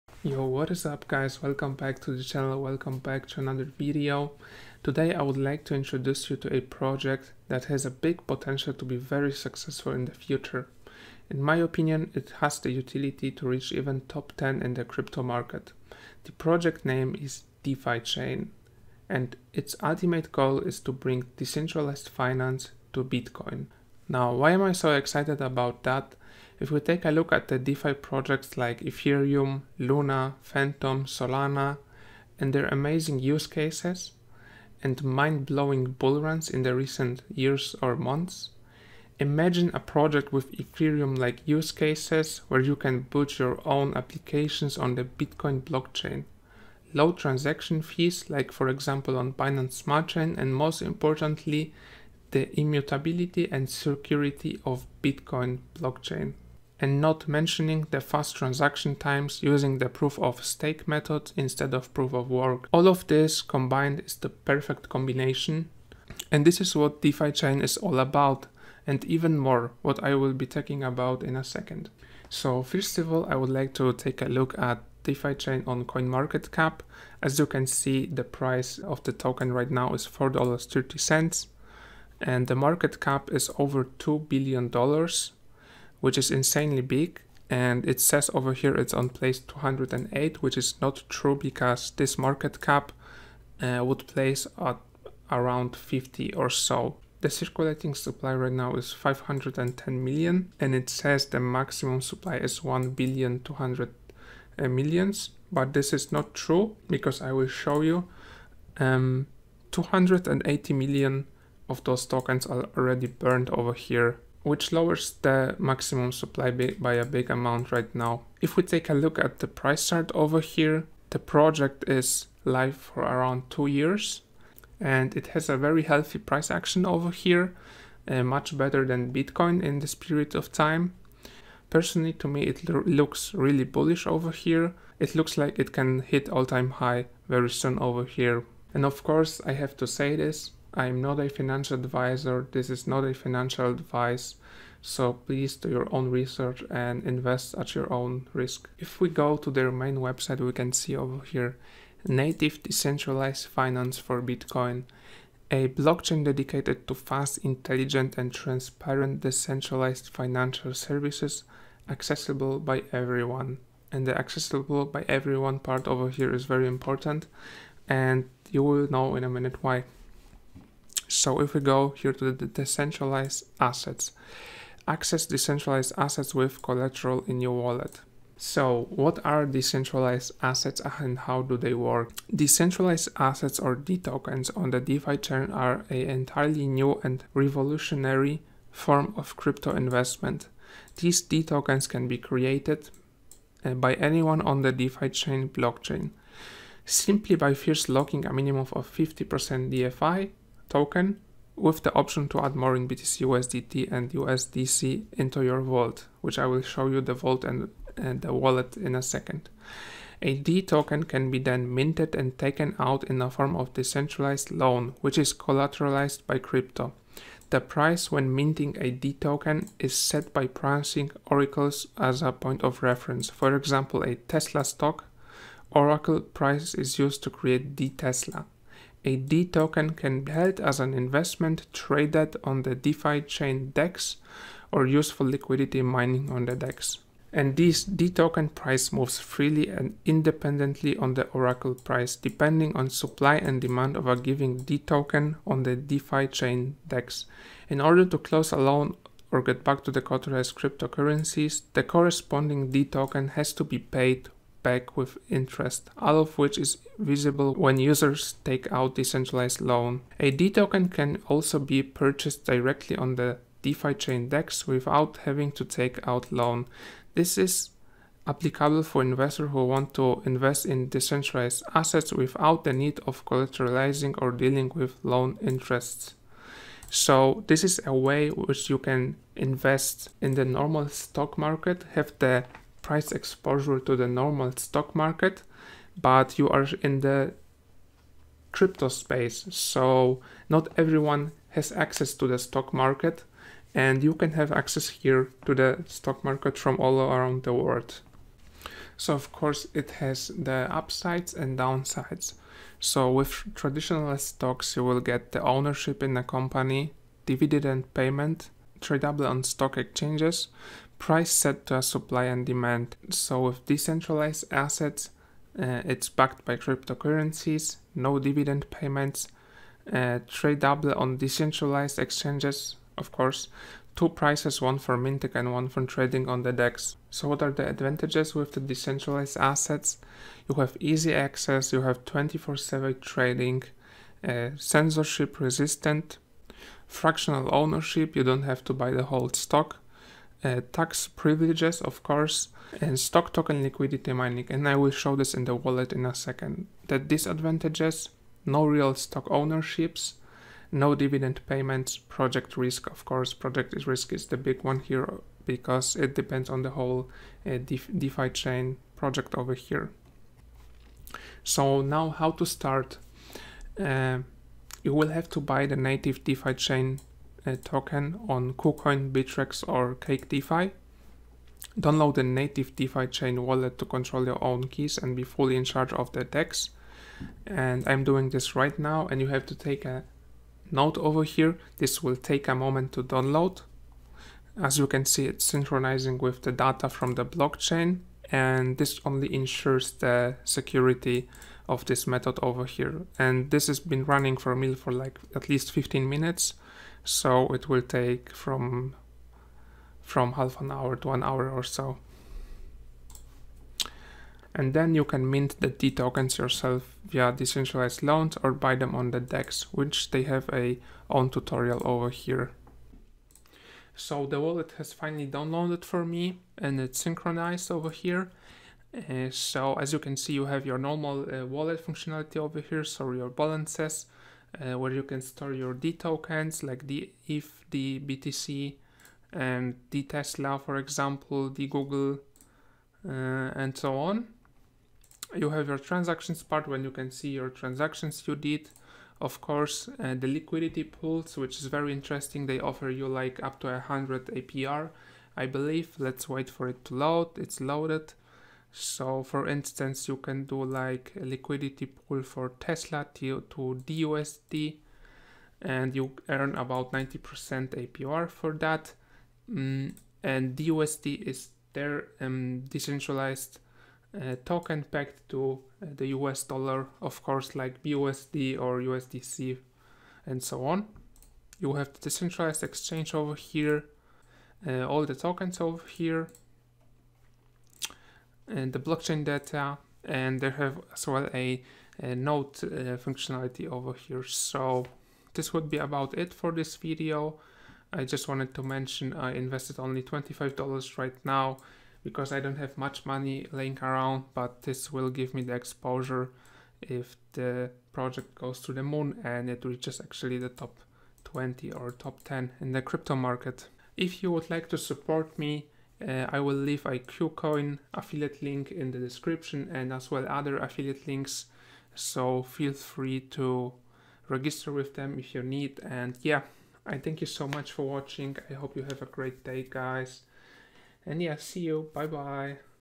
DefiChain ($DFI) - Decentralized Finance for Bitcoin - EXPLAINED
Hi in todays video im talking about DeFiChain ($DFI) a blockchain dedicated to fast, intelligent and transparent decentralized financial services, accessible by everyone build on the BITCOIN network.